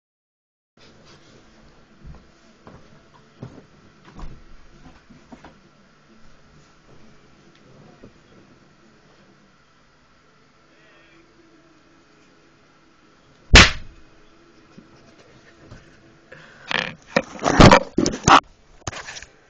very annoying moth
This Is What Happens When Theres A Moth In My Room